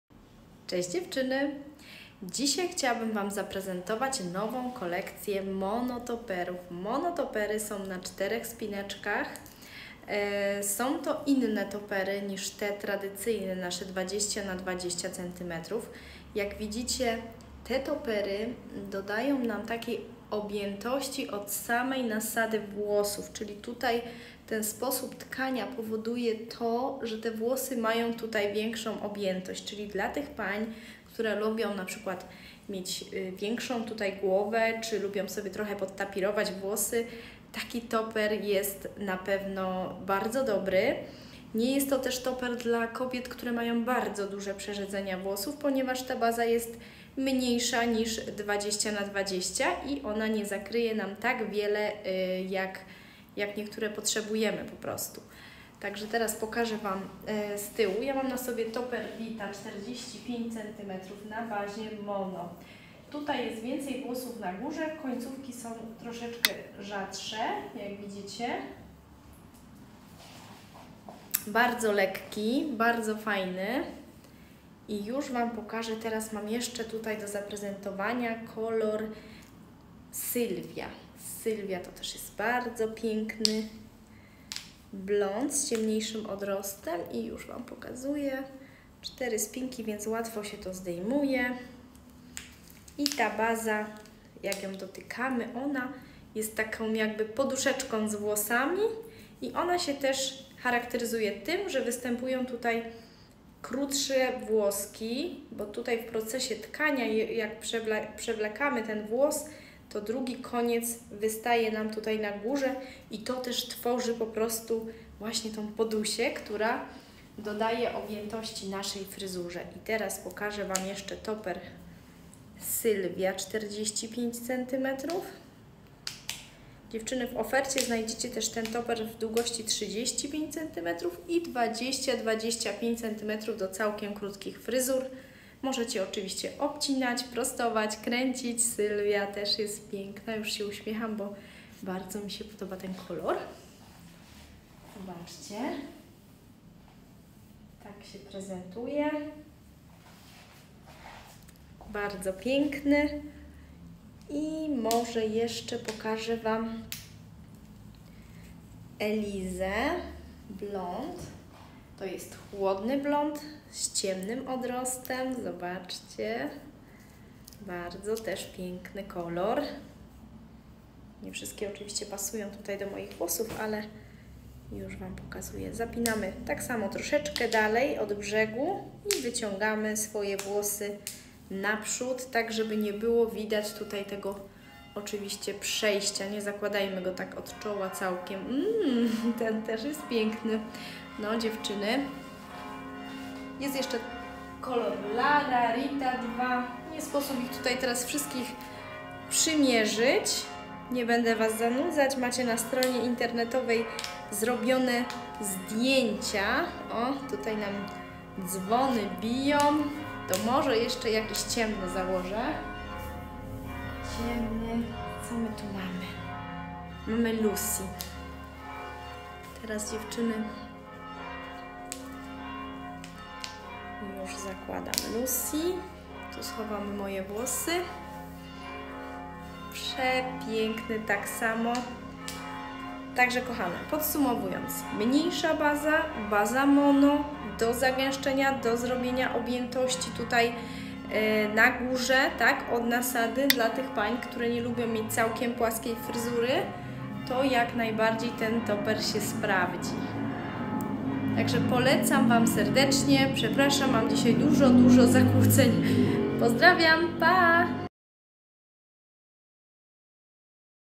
HairLuxpl Mono toppery 45 cm porównanie kolorów
Cześć Kochane! 🌟
Z radością przedstawiam Wam naszą nową kolekcję mono topperów! HAIRLUX🎉
Na filmie możecie zobaczyć toppery:
✨ Rita 45 cm mono
✨ Sylwia 45 cm mono
✨ Eliza 45 cm
✨ Lucy 45 cm
Nasze toppery wykonane są z najwyższej jakości włosów europejskich, które doskonale zagęszczają i unoszą fryzurę od nasady, dając Wam naturalny i pełny wygląd. 🌿💁‍♀️
Zainteresowane różnymi kolorami? Odwiedźcie naszą stronę
Zapraszamy do oglądania i zakupów! 🛍️